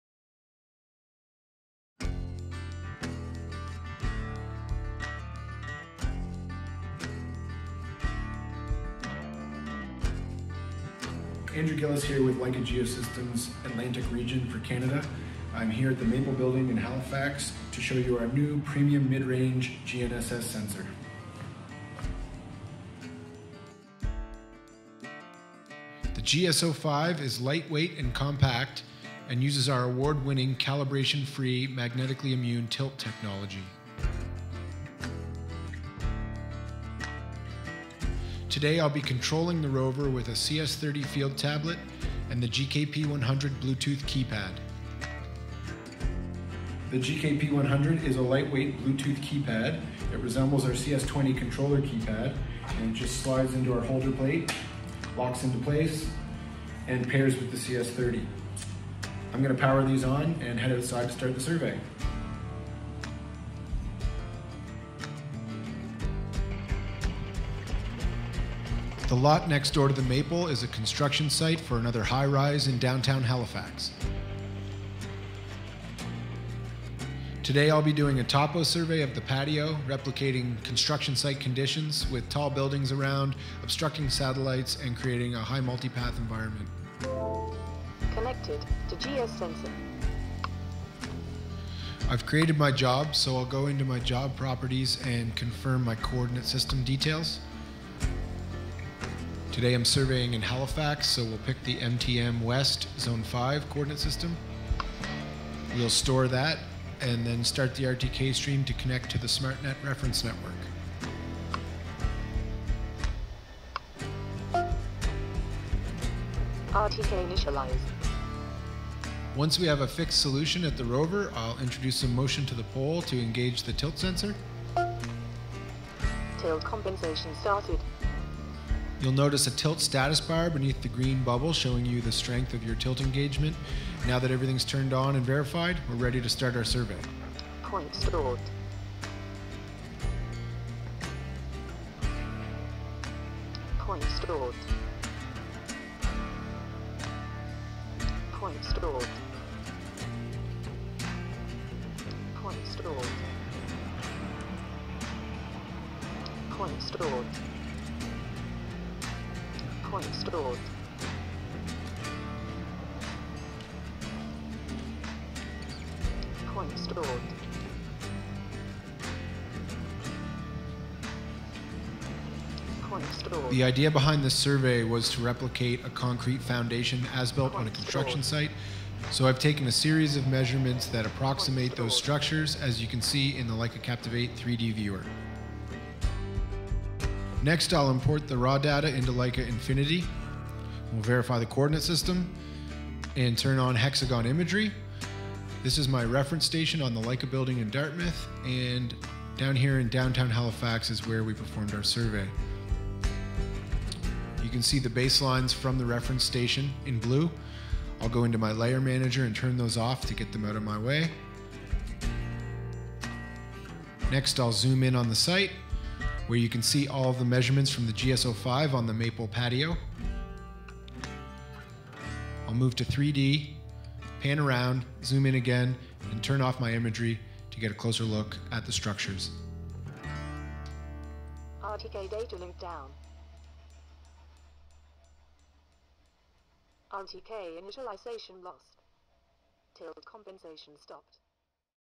Leica Geosystems GS05 High Precision GNSS Sensor
Performing a Topo Survey in Downtown Halifax with the GS05 Receiver, CS20 Field Tablet, GKP100 Keypad, and HxGn SmartNet Cellular Network Satellite Corrections